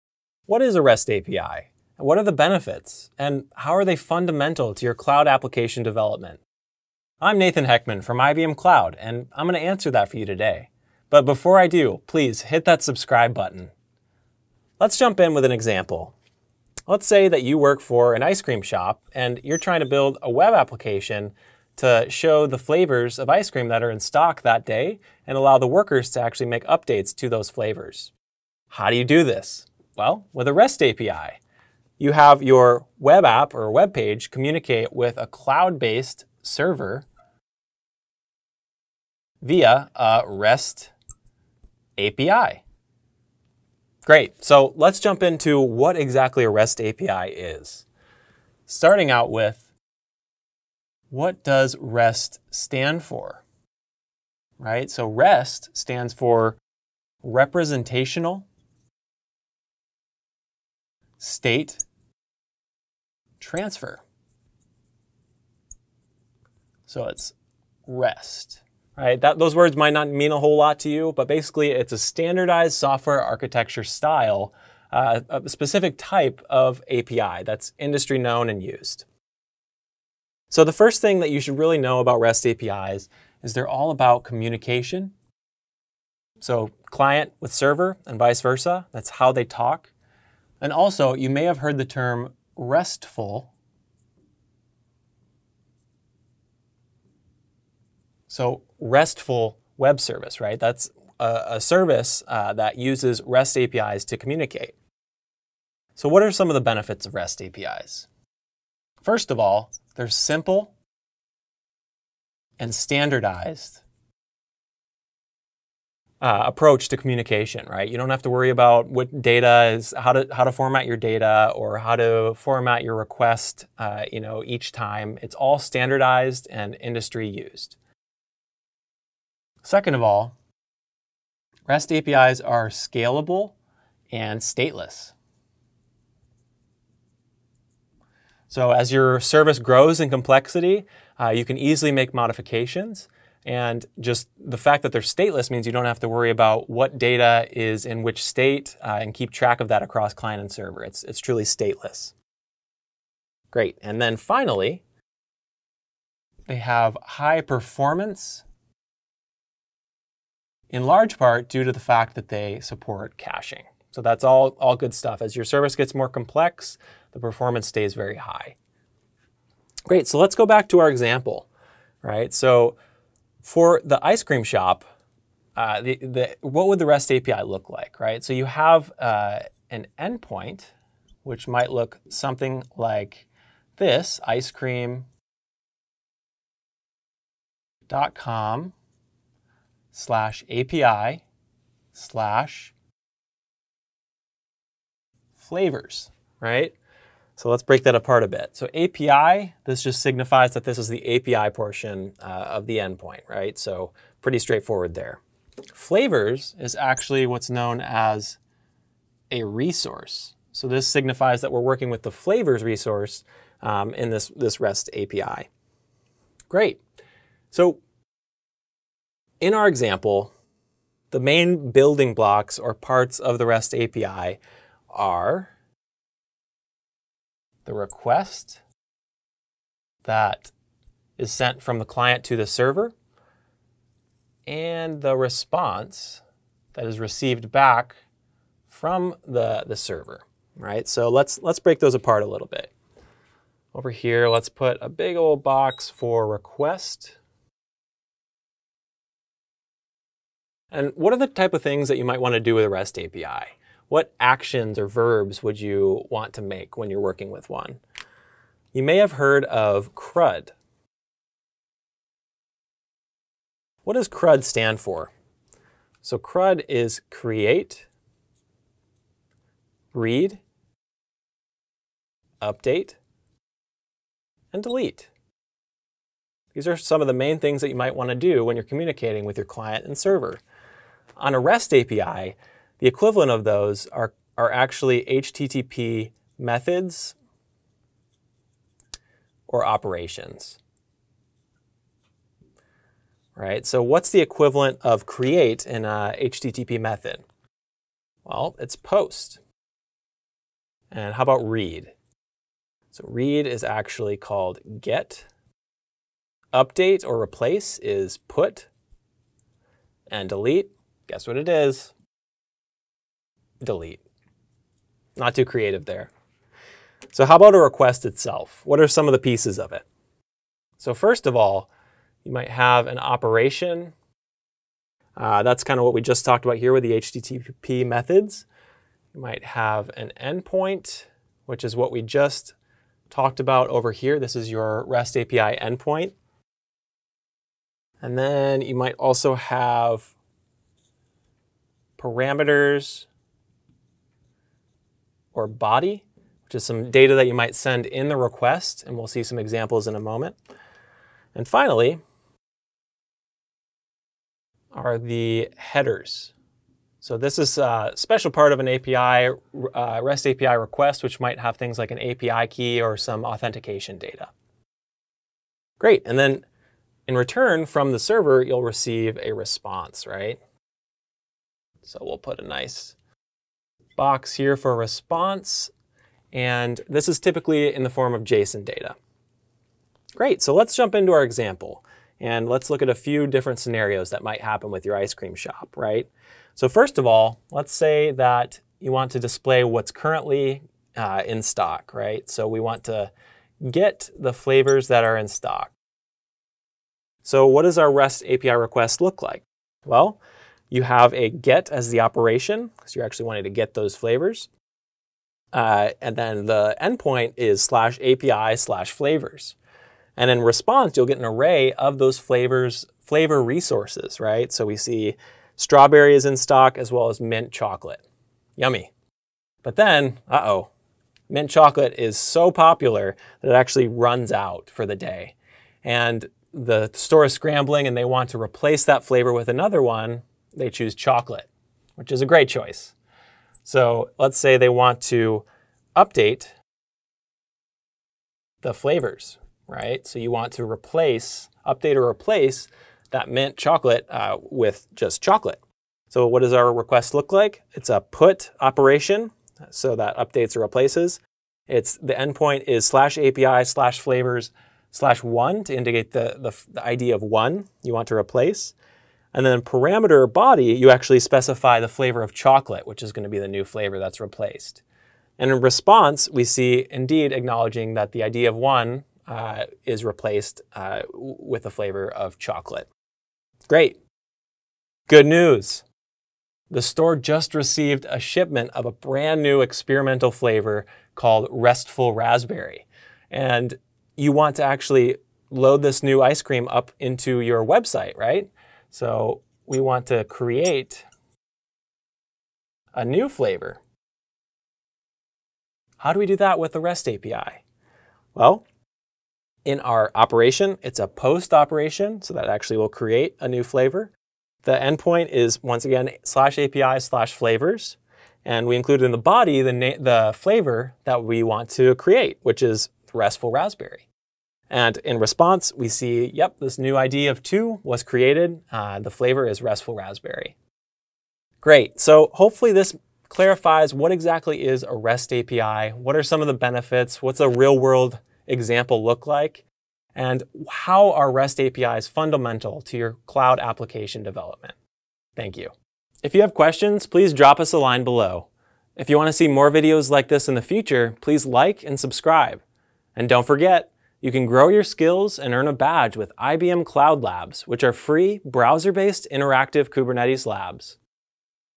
What is a REST API?
What is a REST API? What are the benefits and how are they fundamental to your cloud application?

In this lightboard video, Nathan Hekman with IBM Cloud, answers these questions and much more as he visually shows the benefits a company can gain with using REST API.

Read SmartPaper to learn how to unlock the full potential of your APIs